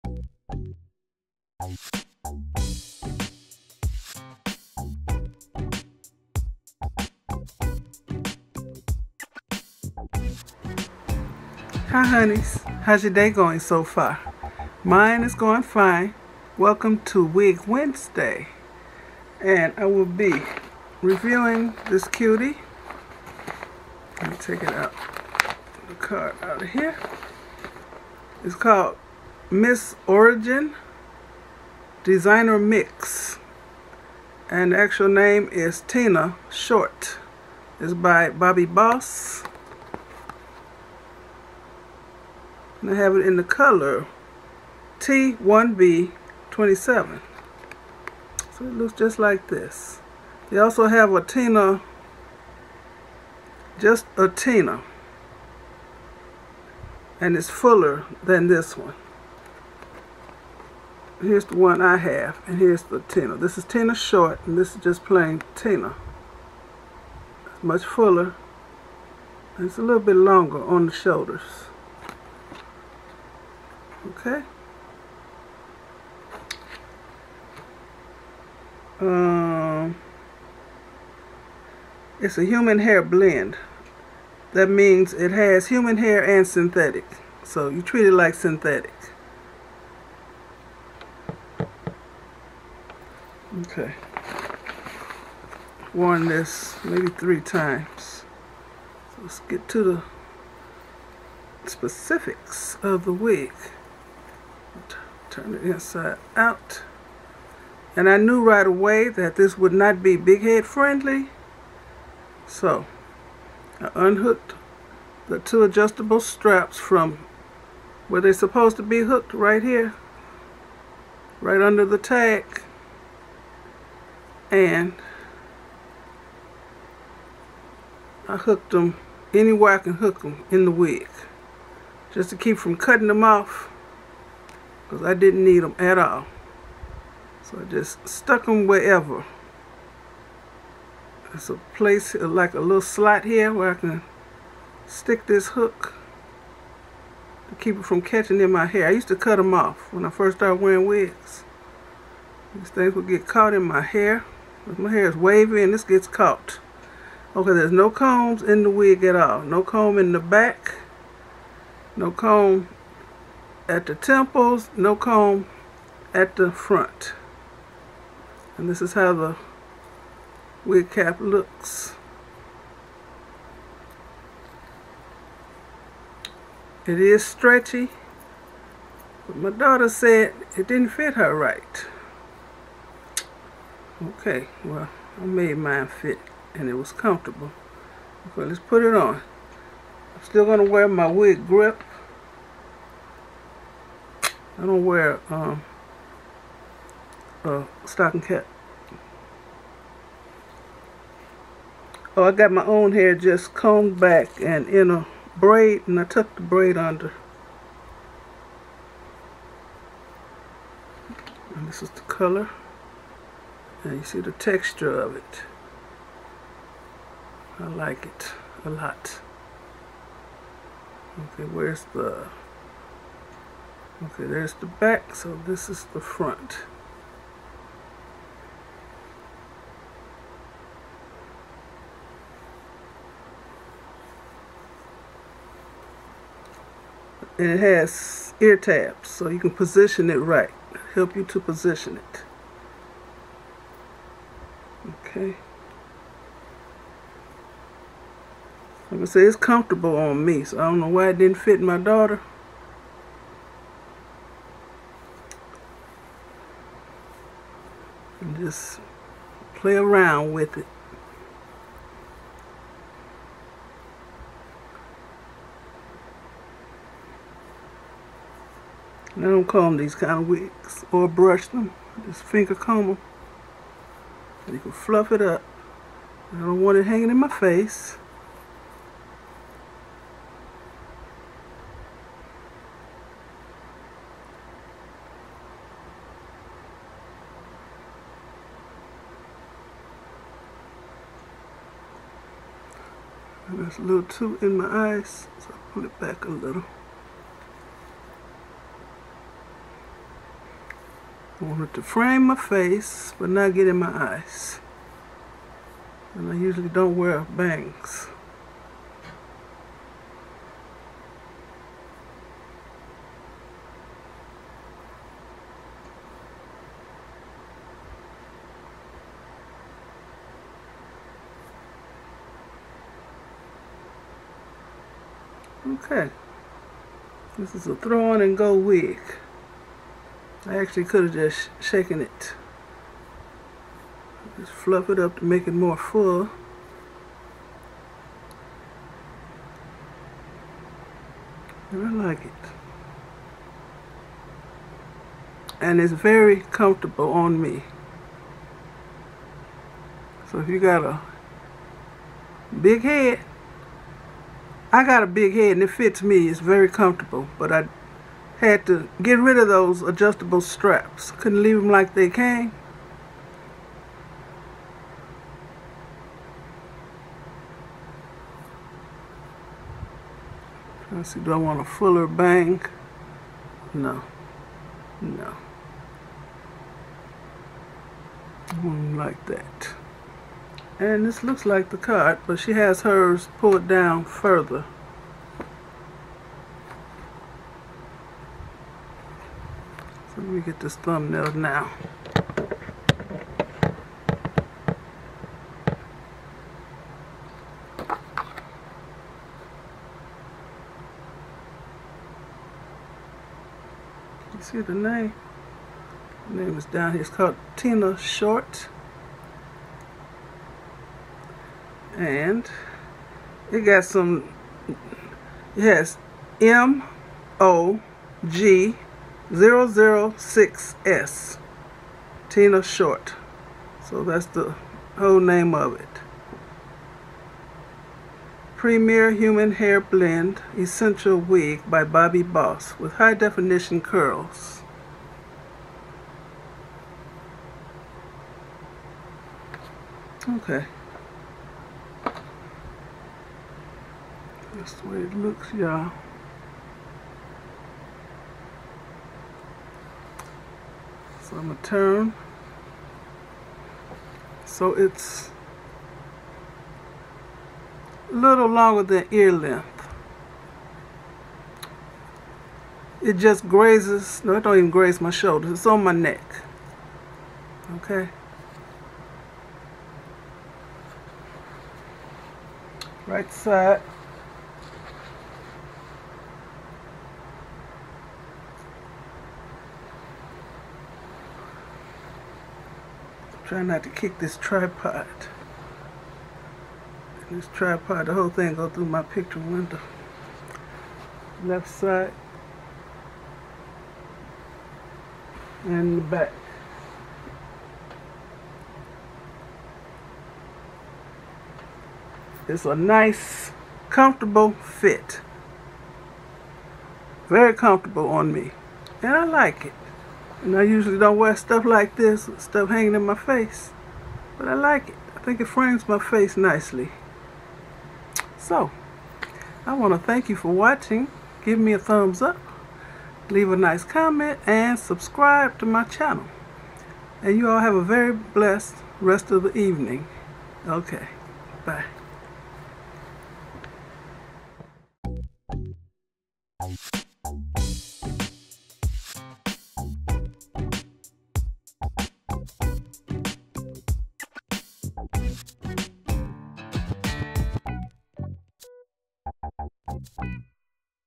Wig Wednesday/Bobbi Boss TINA SHORT/color T1B/27/ Vlogmas 2021 Day #30 - #3202
It's a curly type afro with bangs. Please thumbs UP!💋
💋Bobbi Boss TINA SHORT
💋Wig Grips no lace
💋Wig Grip with Lace
💓MAILING ADDRESS:
 MzBrazell 20650 S. Cicero Ave. #2153 Matteson, Illinois 60443